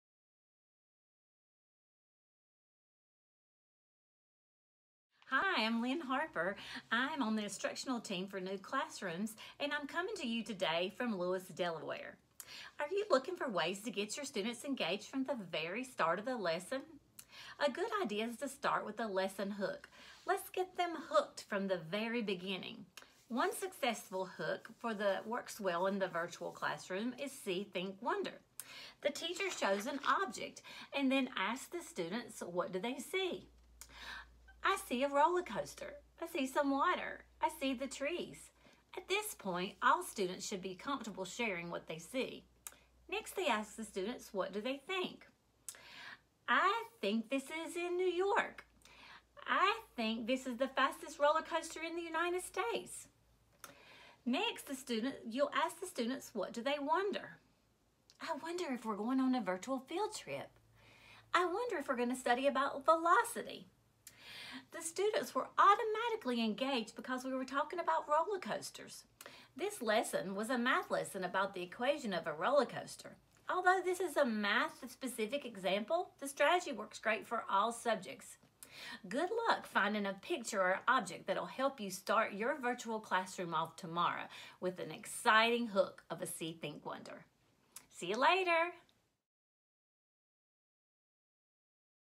Timely Teacher Tips: Icebreakers, Pt. 2
“In this Timely Teacher Tip, New Classrooms Director of Instructional Support Lyn Harper shares another icebreaker idea to help set the tone in your virtual classroom.”